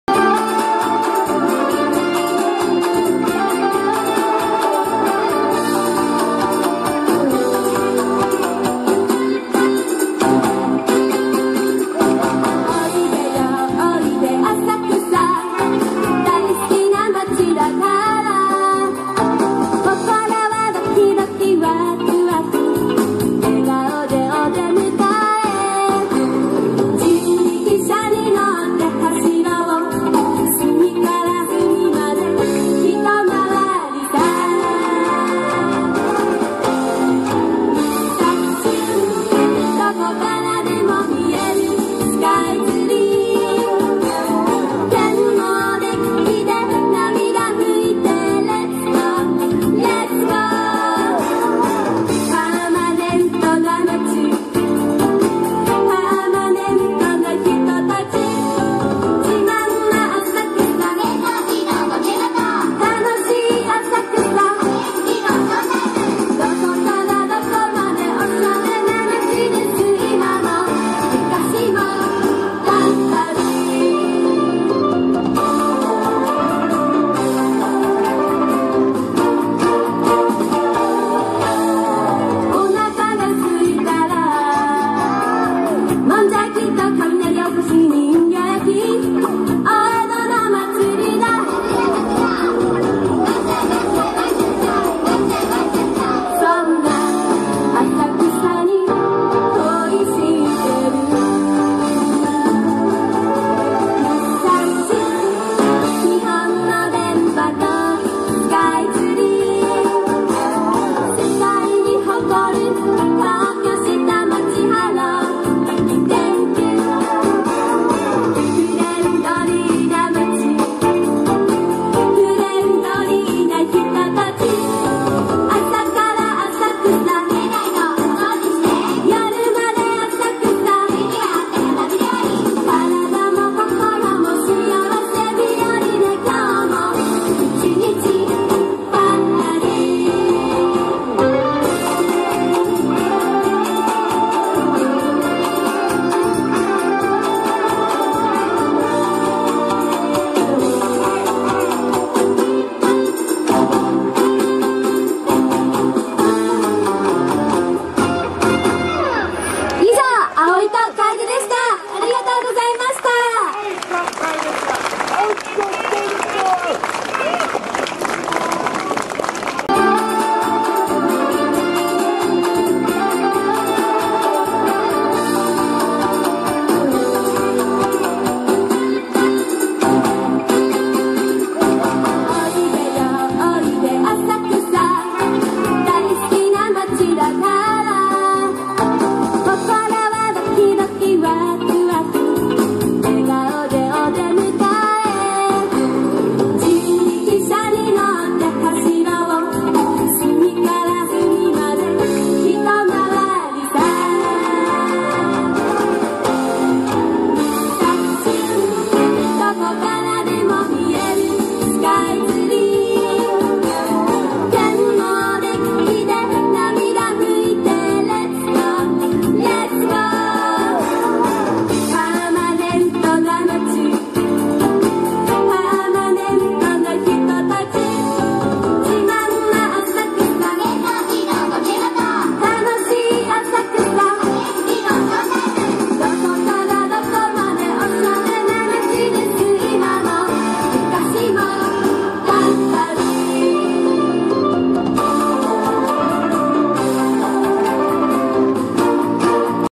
ฟังเพลงสองสาวญี่ปุ่น ช่วง full bloom เทศกาลซากุระ 2019
เที่ยวญี่ปุ่นเอง ฟังเพลง เทศกาลซากุระ ช่วง full bloom เทศกาลซากุระ 2019
เที่ยว
ริมแม่น้ำ
เทศกาลชมดอกซากุระ หรือ ฮานามิ Hanami